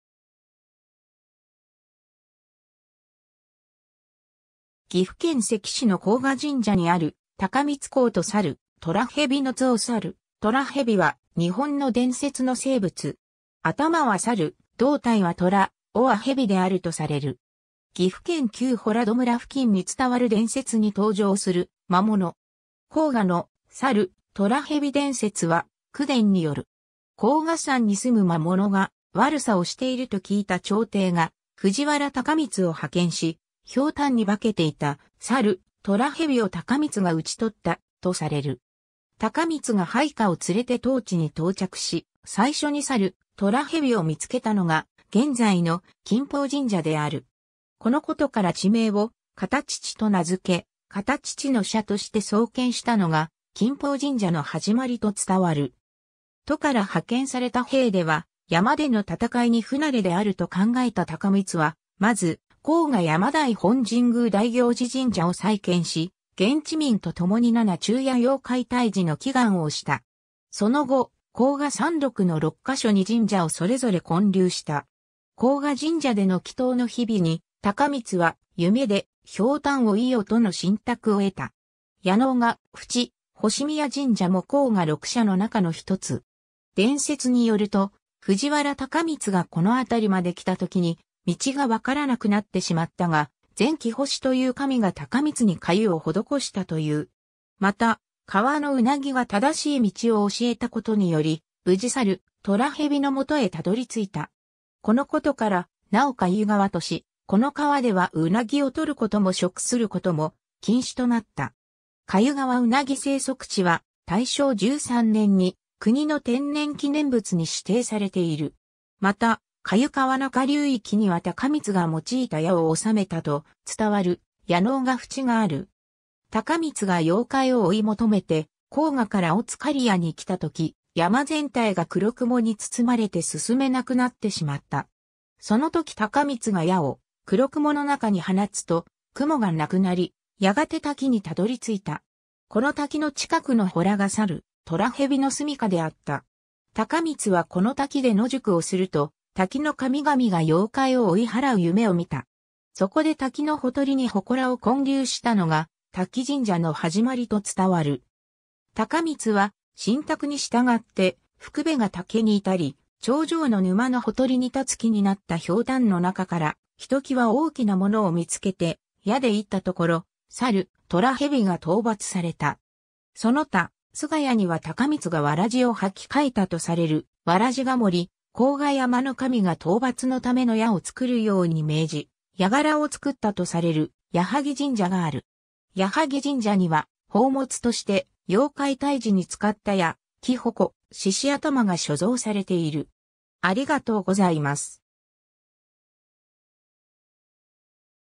さるとらへび
神話・伝説の合成獣
岐阜県関市の高賀神社にある「高光公とさるとらへび」の像 さるとらへび（猿虎蛇）は日本の伝説の生物。
頭は猿、胴体は虎、尾は蛇であるとされる。
 岐阜県旧洞戸村（現関市）付近に伝わる伝説に登場する魔物。
高賀の「さるとらへび伝説」は口伝による。
 高賀山に住む魔物が悪さをしていると聞いた朝廷が藤原高光を派遣し、瓢箪に化けていたさるとらへびを高光が討ち取った、とされる。
 高光が配下を連れて当地に到着し、最初にさるとらへびを見つけたのが現在の『金峰神社 (美濃市)』である。
このことから地名を『形知』（現・片知）と名づけ、『形知の社』として創建したのが金峰神社の始まりと伝わる。
 都から派遣された兵では山での戦いに不慣れであると考えた高光は、先ず『高賀山大本神宮大行事神社（現・高賀神社）』を再建し、現地民と共に七昼夜妖怪退治の祈願をした。
その後、高賀山麓の六ヶ所に神社（高賀六社）をそれぞれ建立した。
高賀神社での祈祷の日々に、高光は夢で「瓢箪を射よ」との神託を得た。
 矢納ヶ渕 『星宮神社』も高賀六社の中の一つ。
伝説によると、藤原高光がこの辺りまできたときに、道がわからなくなってしまったが、善貴星という神が高光に粥を施したという。
また、川の鰻が正しい道を教えたことにより、無事さるとらへびの元へ辿り着いた。
このことから、名を粥川とし、この川では鰻を採ることも食することも禁止となった。
「粥川ウナギ生息地」は大正13年（1924年）に国の天然記念物に指定されている。
また、粥川中流域には高光が用いた矢を納めたと伝わる『矢納ヶ渕』がある。
 高光が妖怪を追い求めて高賀から乙狩谷に来た時、山全体が黒雲に包まれて進めなくなってしまった。
そのとき高光が矢を黒雲の中に放つと雲が無くなり、やがて滝にたどり着いた。
この滝の近くの洞がさるとらへびの棲家であった。
高光はこの滝で野宿をすると、滝の神々が妖怪を追い払う夢を見た。
そこで滝のほとりに祠を建立したのが、『瀧神社』の始まりと伝わる。
 高光は、神託に従って『ふくべヶ岳』に至り、頂上の沼のほとりに立つ木に成った瓢箪の中から、ひときわ大きなものを見つけて矢で射た所、さるとらへびが討伐された。
 その他、菅谷には高光が草鞋を履き替えたとされる『草鞋が森』、高賀山の神が討伐のための矢を作るように命じ、矢柄を作ったとされる『矢作神社』がある。
矢作神社には宝物として妖怪退治に使った矢、木鉾、獅子頭が所蔵されている。